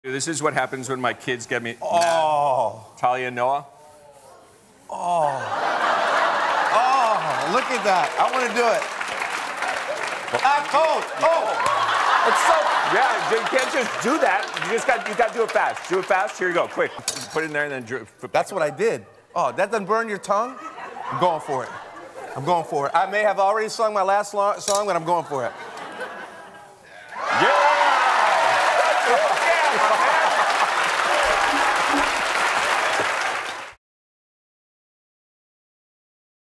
Chicago Chef Phillip Foss Cooks with Liquid Nitrogen
You've never seen food like this!  Harry tries out some innovative new dishes with Phillip Foss.